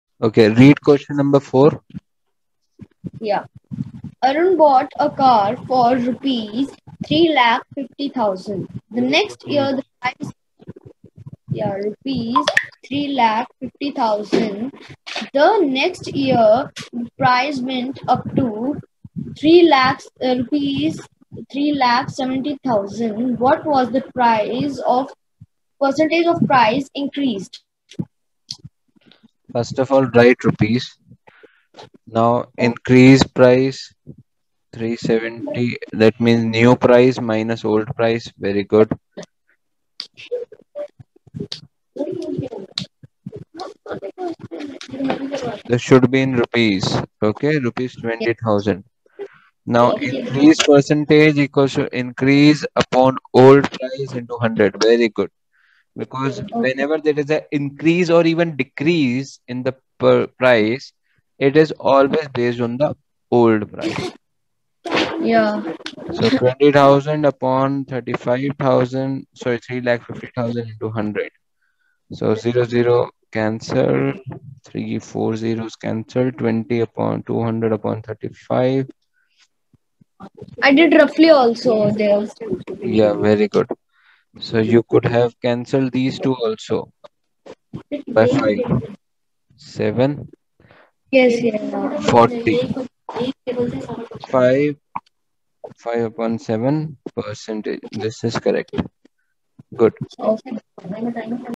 Ex 8.3 Q4 Arun bought a car for Rs3,50,000 next year the price went to Rs3,70,000. Percentage inc?
Class 7th,
Comparing Quantities,
Chapter 8,
Ex 8.3,
Q4,
NCERT Solution,
Arun bought a car for Rs350000,
The next year,
the price went upto Rs370000,
What was the Percentage of price increase?